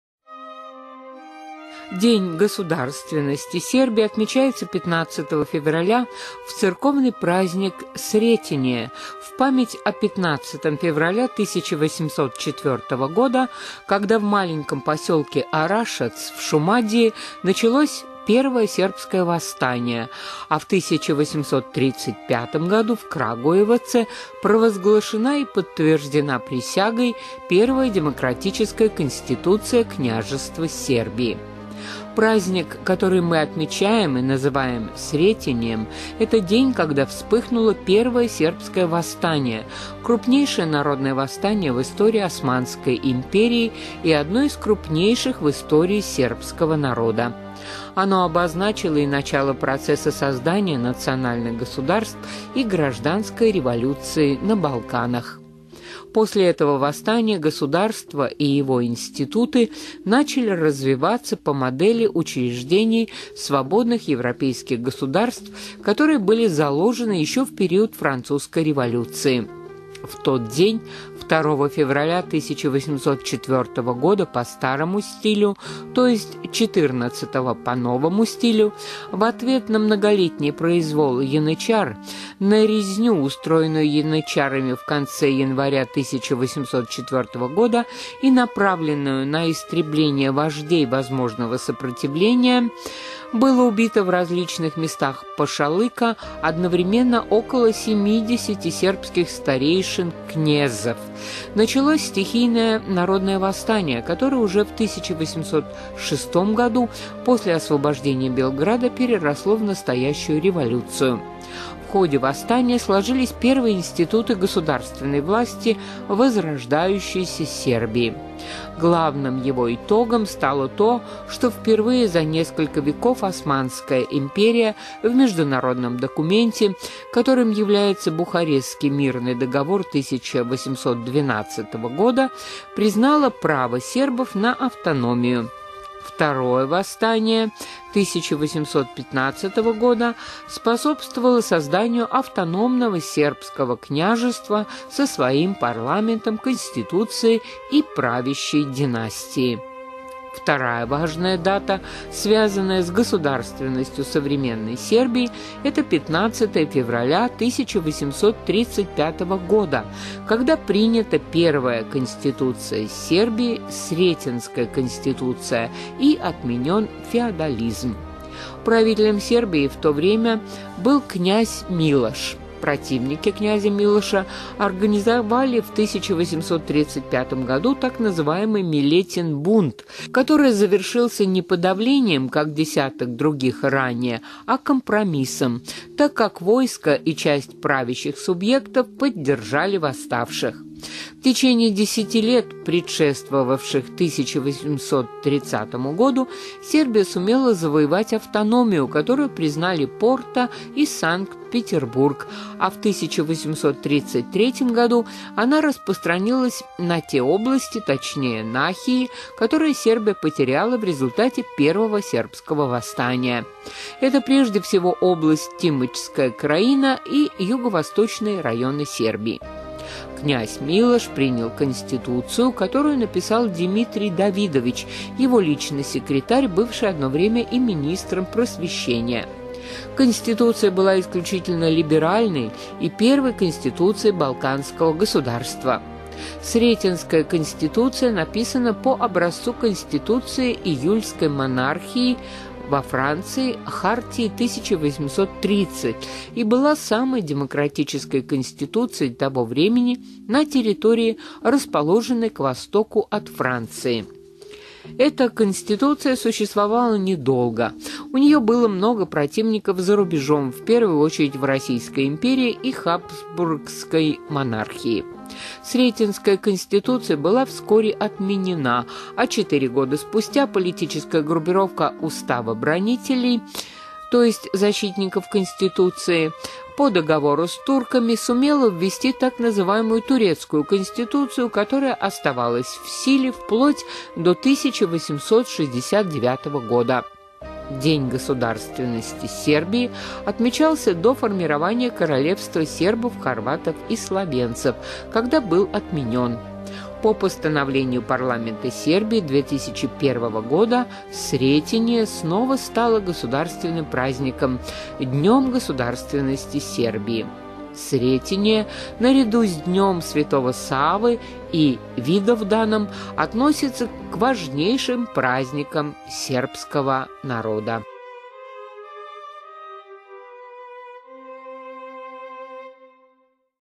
Сретение – традиция свободолюбия и героизма
День государственности Сербии отмечается 15 февраля, в церковный праздник Сретение в память о 15 февраля 1804 года, когда в маленьком поселке Орашац, в Шумадии, началось  Первое сербское восстание, а  в 1835 году в Крагуеваце провозглашена и подтверждена присягой первая демократическая Конституция Княжества Сербии.